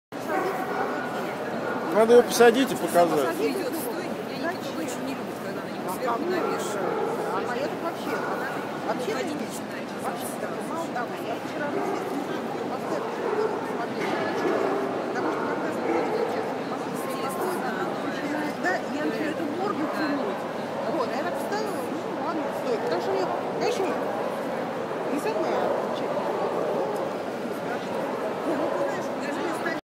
Eurasia-2, Moscow CACIB show 25.03.2012 Puppy Class Males p.1
judge: Giuseppe Colombo Manfroni (It)

Кобели/Males Класс щенков/Puppy class
3 ОП / 3 v.g.
TRULY YOURS WILD KING
 black/tan
Truly Yours Spanish Obsession
Truly Yours Royal Romance
Br.Hansen, Ow. M. Zueva

Б.П./ v.p. - АДМИРАЛ'С ШОУ САННИ ВАЙНД
 chocol.
ADMIRAL`S SHOU BRAVE TIGER
ADMIRAL`S SHOU STAR IN THE NIGHT
зав. + вл. Ковальчук
4 ОП / 4 v.p.
GORDOST URALA GUCCI HIGH TECH "Гуччи "
 choc&tan
ADMIRAL'S SHOU BECAUSE I LOVE YOU
ANASTASIA ANDROS D-JESSY JAMES br. Медведева,
own. Белоусова, Дегтярск

ЖЕНЕВЬЕВА ХОКУС-ПОКУС «Тема»
 9.07.11, black/tan
Admiral's Show Because I Love You, "Лева"
Zheneveva PARTY BEYOND THE STARS, "Яся"

2 ОП / 2 v.p.
ЛЕТЯЩАЯ ПТИЦА ЛУУК ЭС ГОД
ЩК,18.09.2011 , клеймо: BEP 149, b/w
MY-IDA-HO HALF TIME
MY-IDA-HO LACE N RUFFLES Зав. Худякова,
вл. Мамаева

1 ОП / 1 v.p., ЛУЧШИЙ ЩЕНОК КОБЕЛЬ / Best puppy male
СИАММ САМУРАЙ ФО ВИВАТ ФОРЕВЕ
РКФ 3043431, 17.07.11, клеймо: AST 66, bl/t
Siamm Incredible Son Of The Future
Admiral's Show What a Wonderful Girl завСемьюшкин М
вл.Вереш Е.